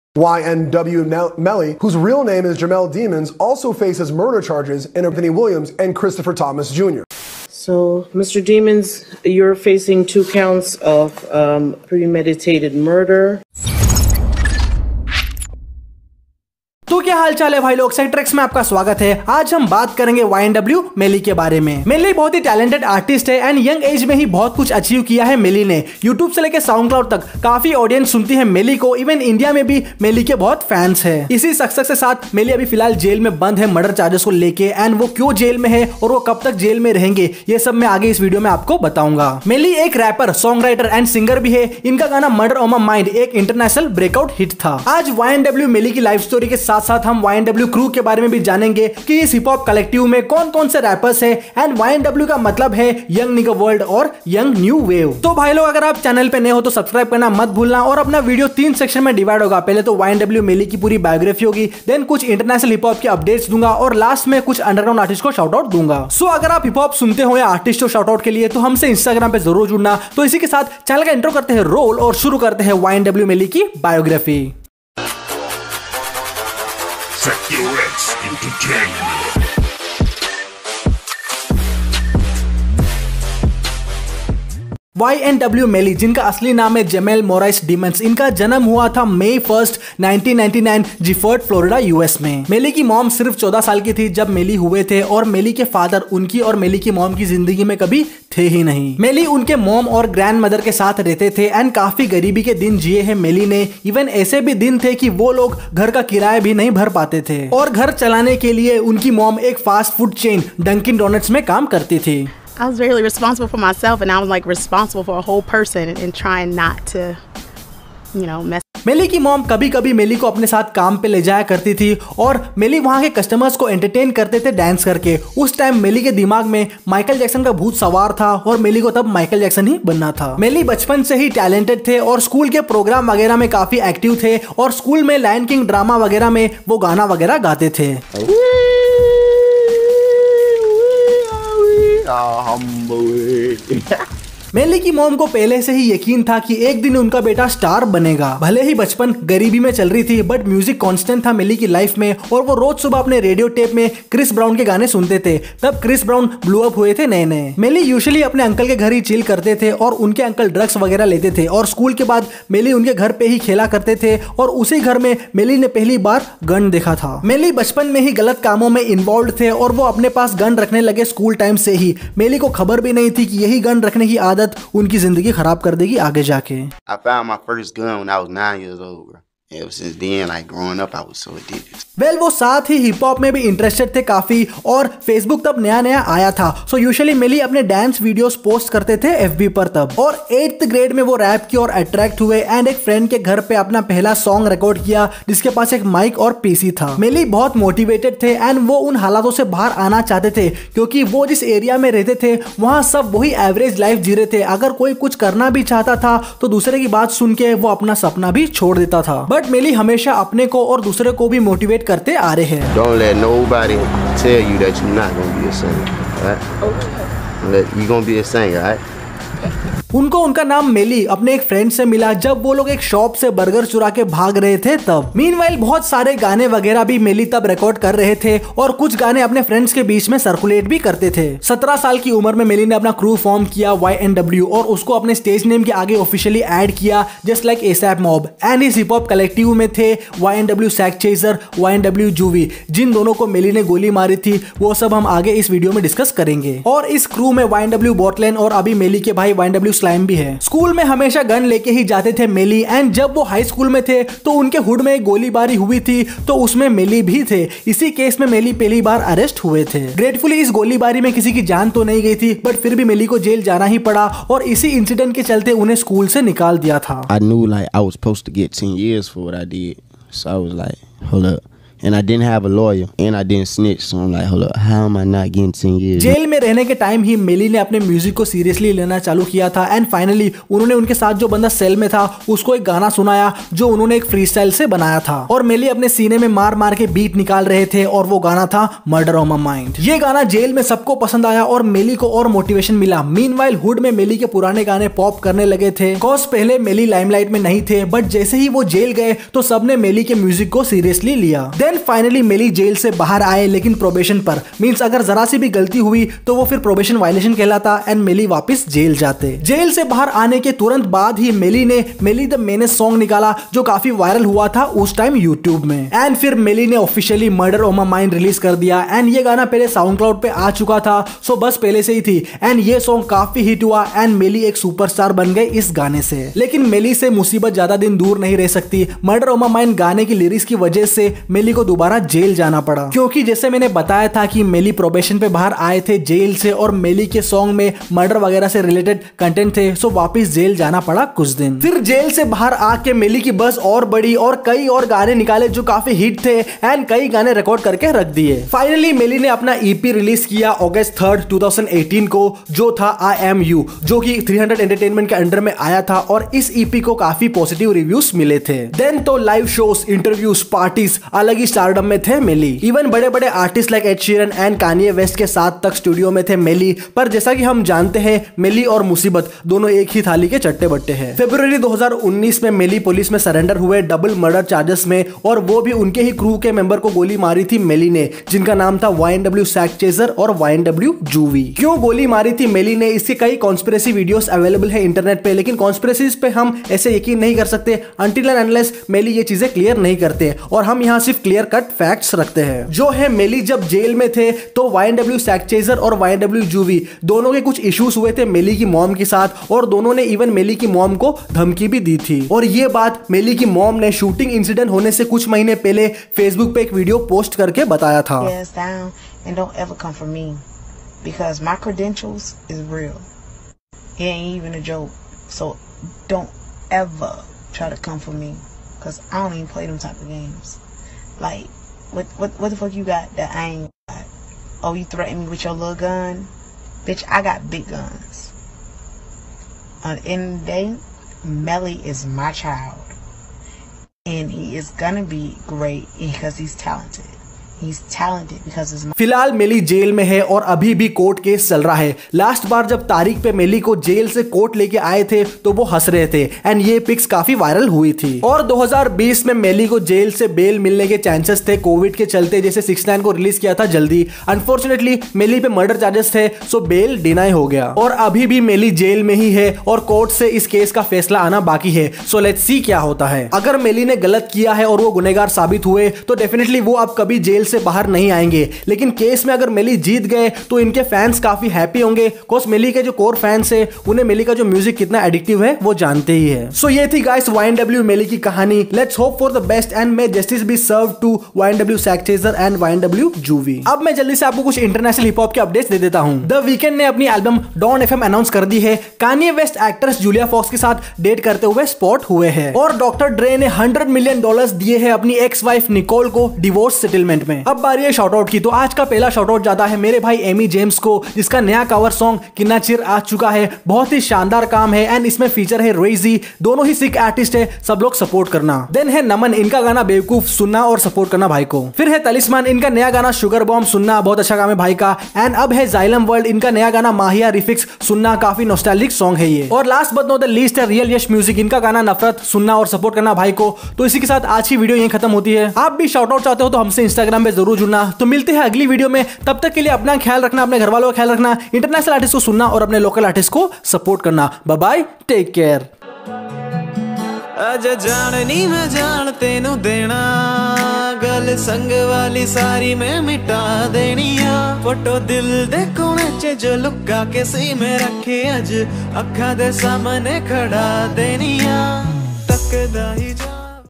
YNW Melly Life Story in Hindi (LATEST) | Hip Hop कहानी Ep. #47 | YNW FULL BIOGRAPHY
Ynw melly Hindi Biography.

Do support and share our channel with your Hip Hop Friends .

If you're reading this,I just wanna let you  know that you will achieve all your dreams one day :)